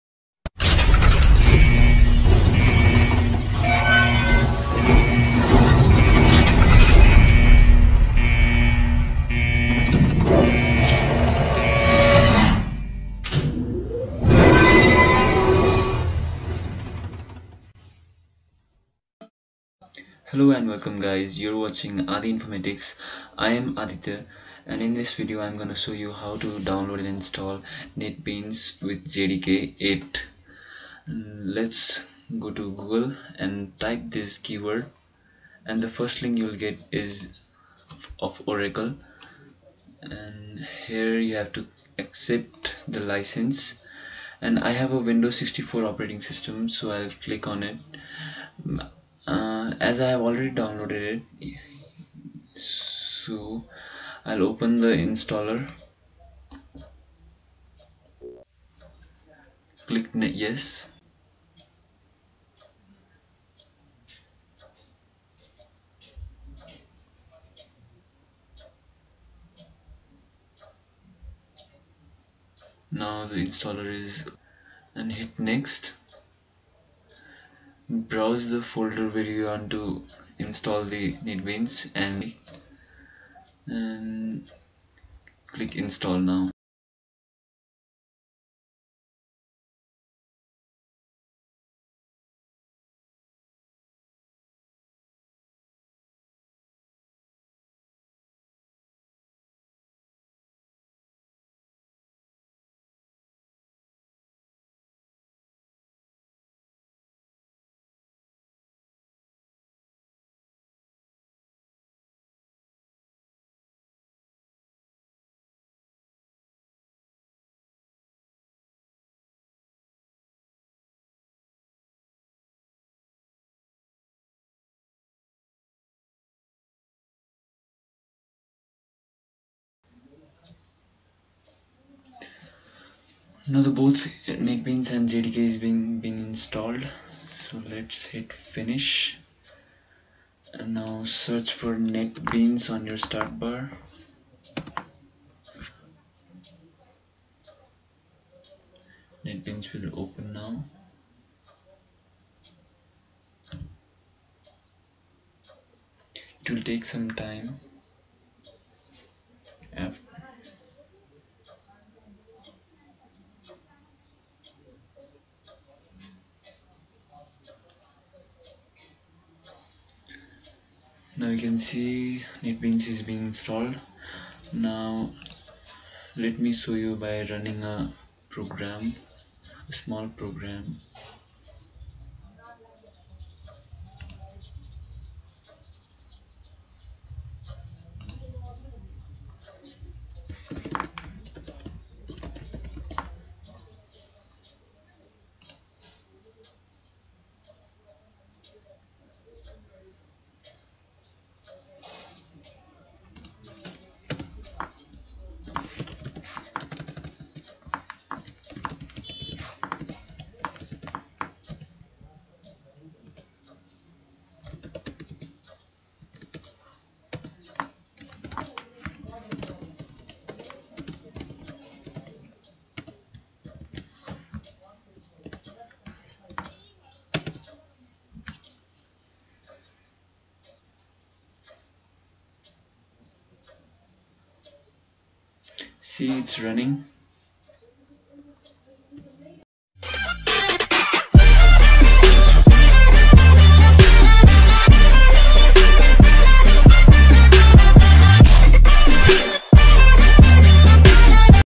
How to install Netbeans 8.2 and jdk 8 on Windows 10 | Latest 2020 | JAVA
In this video, I have shown how to install Netbeans IDE 8.2 with JAVA JDK 8. Netbeans is the most popular and outstanding IDE for different languages mainly for JAVA.